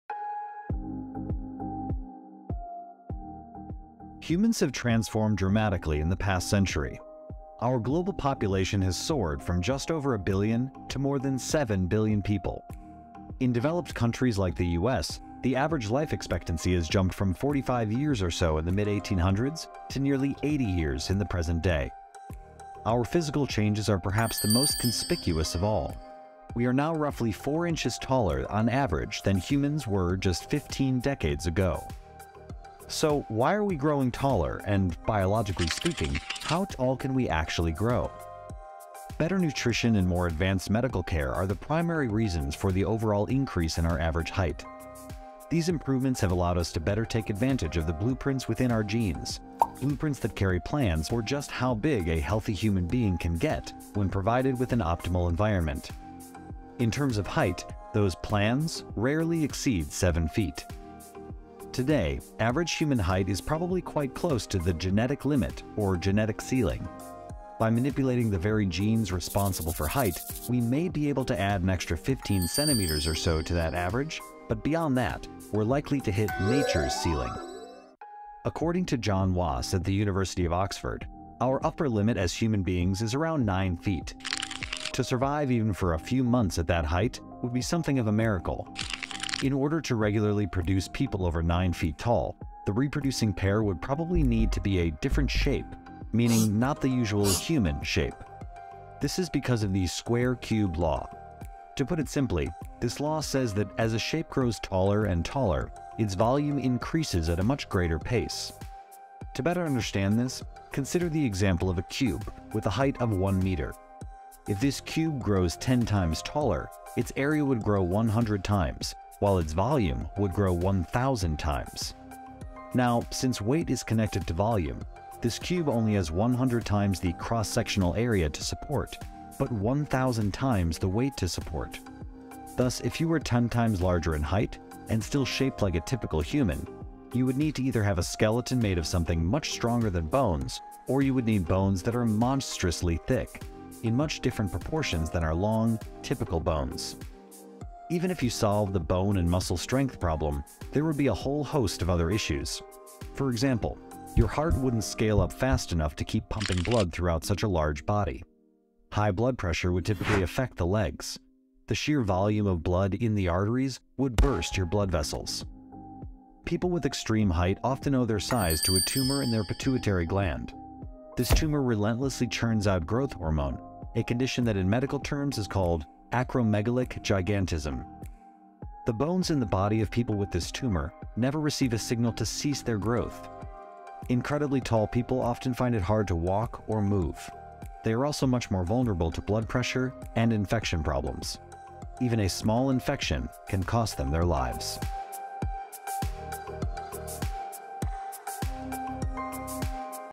How Tall Can Humans Really Get?
Our upper limit for height, or genetic limit, is around 9 feet. To survive even for a few months at that height would be a bit of a miracle. In order to regularly produce people over 9 feet tall, the reproducing pair would probably need to be a different shape—not the usual human shape. This is because of the square-cube law. To put it simply, this law says that as a shape grows taller and taller, its volume increases at a much greater pace.

Extreme height is often caused by a pituitary gland tumor that produces excess growth hormone, known as acromegalic gigantism. The tumor prevents the release of sex hormones that normally limit bone growth, resulting in continuous growth. Very tall individuals face difficulties in movement and increased risks of blood pressure issues and infections, which can be fatal.